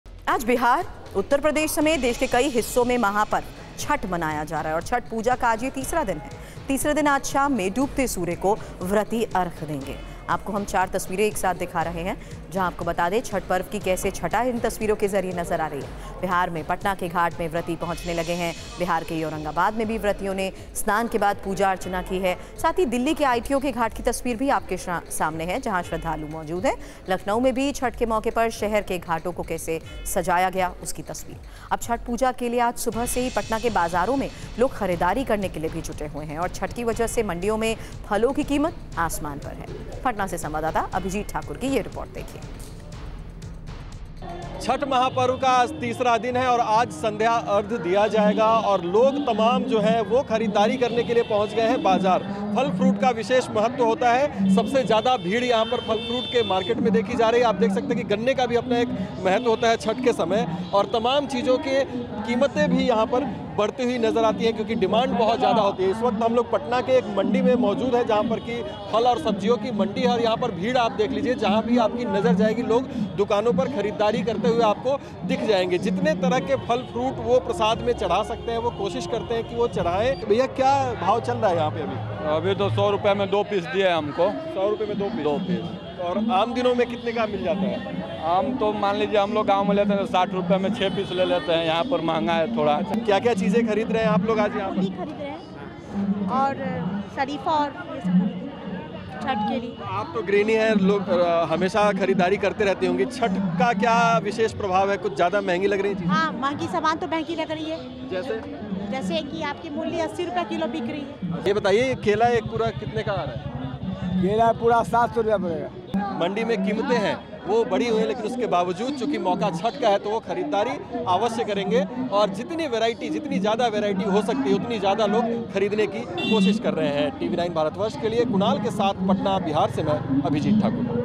Chhath Puja: आज बिहार,उत्तर प्रदेश समेत देश के कई हिस्सों में महापर्व छठ मनाया जा रहा है
आज बिहार,उत्तर प्रदेश समेत देश के कई हिस्सों में महापर्व छठ मनाया जा रहा है...छठ पूजा का आज तीसरा दिन है... तीसरे दिन आज शाम में डूबते सूर्य को व्रती अर्घ्य देंगे.. आपको चार तस्वीरों के जरिए छठ पर्व की छटा दिखा रहे हैं...बिहार में पटना के घाट में व्रती पहुंचने लगे हैं... बिहार के ही औरंगाबाद में व्रतियों ने स्नान के बाद पूजा की...दिल्ली में ITO के घाट पर भी श्रद्धालु पहुंचने लगे हैं... लखनऊ में छठ के मौके पर शहर के घाटों को सजाया गया है..